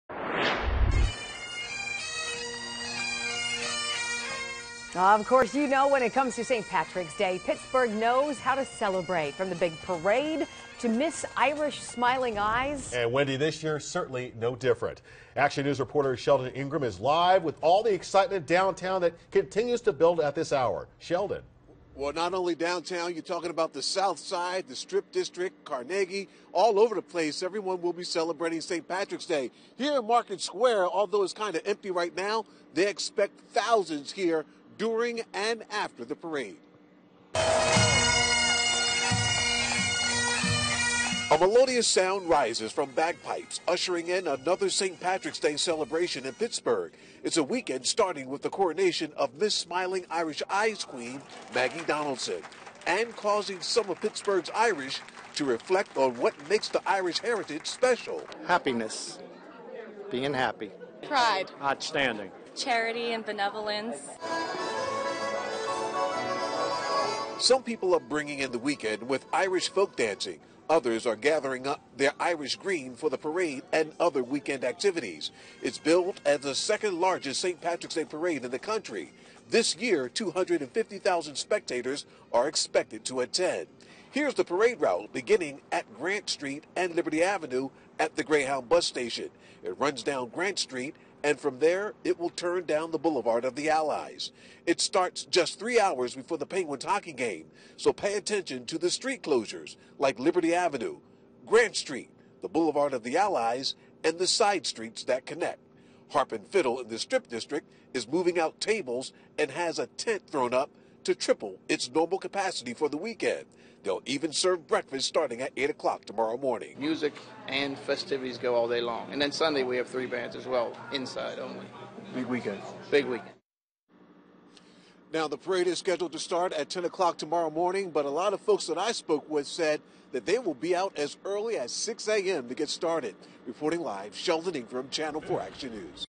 Pittsburgh gets ready for St. Patrick's Day celebrations
Channel 4 Action News' Sheldon Ingram takes a look at how Pittsburgh is getting ready to celebrate St. Patrick's Day.